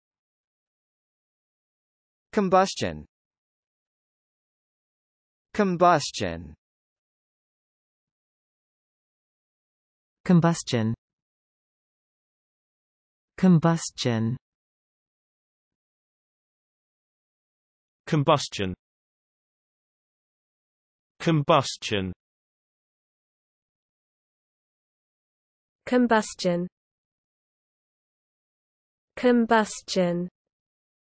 How to pronounce combustion
Learn how to say Combustion in English correctly with TextToSpeech.io free pronunciation tutorials. American & British English pronunciation of male & female voices provided in videos. Check them out!

    Combustion definitions:
    (n): the act of burning something; "the burning of leaves was prohibited by a town ordinance"
    (n): a process in which a substance reacts with oxygen to give heat and light
    (n): a state of violent disturbance and excitement; "combustion grew until revolt was unavoidable"